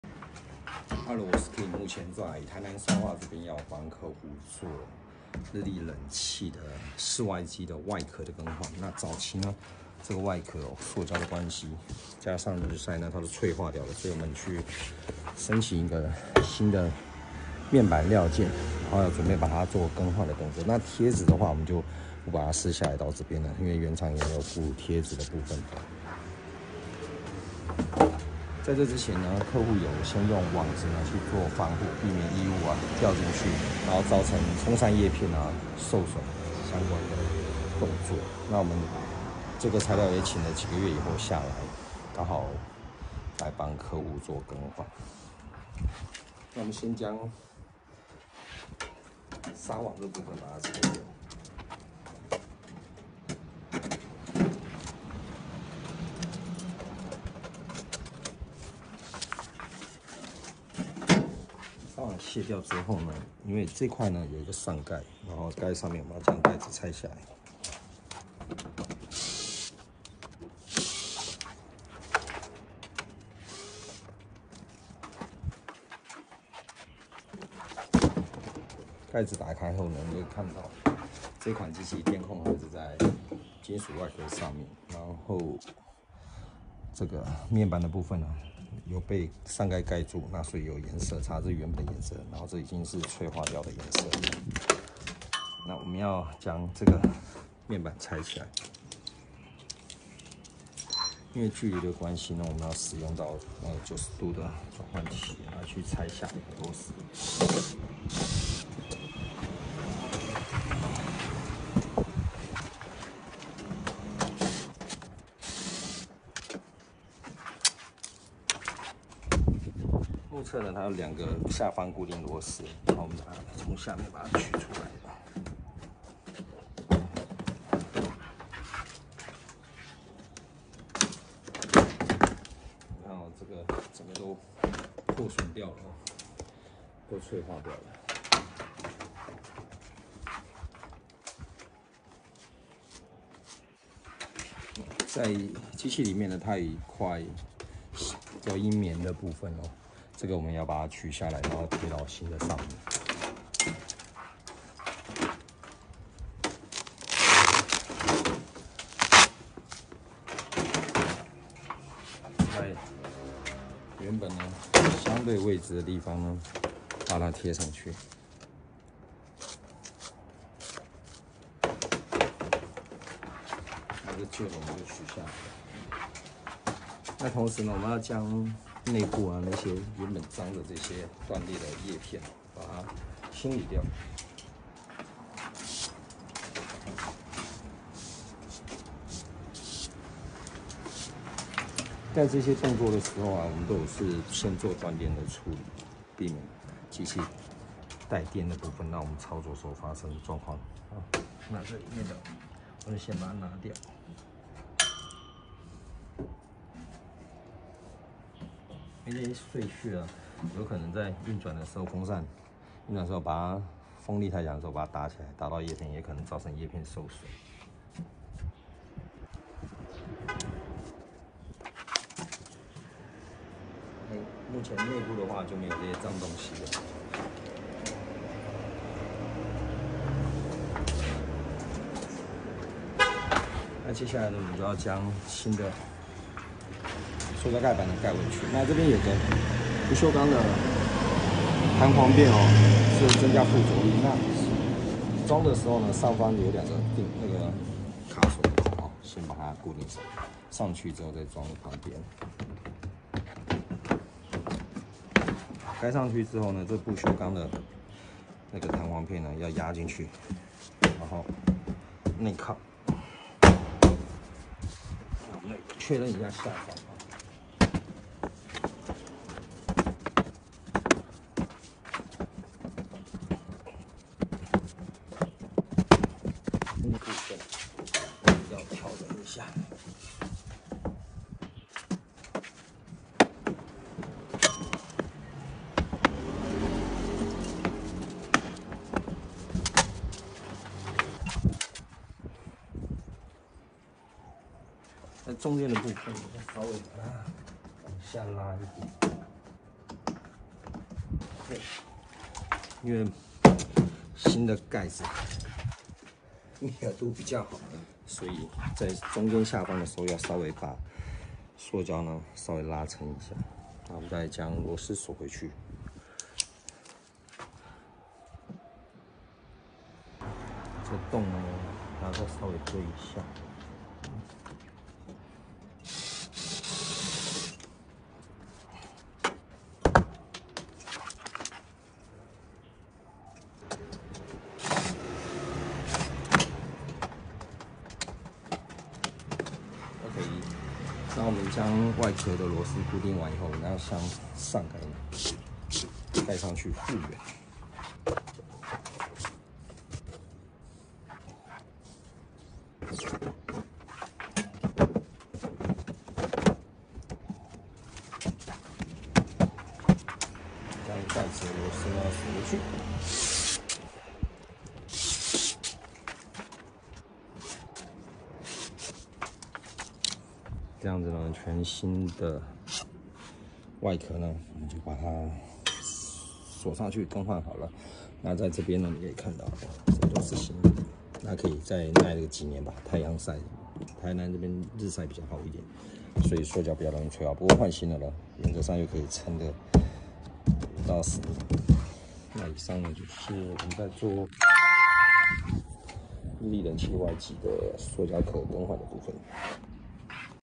冷氣室外機外殼破損嗎？炎炎的夏日長時間的曝曬，造成室外機塑膠零件破損脆化，更可能造成嚴重機器損壞問題產生！透過新的料件更換，確保冷氣運轉功能正常 日立冷氣室外機殼破損更換案例
炎炎的夏日長時間的曝曬
造成室外機塑膠零件破損脆化
更可能造成嚴重機器損壞問題產生！
透過新的料件更換，確保冷氣運轉功能正常
影片為日立冷氣室外機外殼更換的案例分享
如果你有相關的問題
您可以參考這裡！
把冷氣的正確知識分享給更多的人 為自已生活把關
您的支持是我們做影片的動力來源
我們隨時分享最新案例給您，謝謝！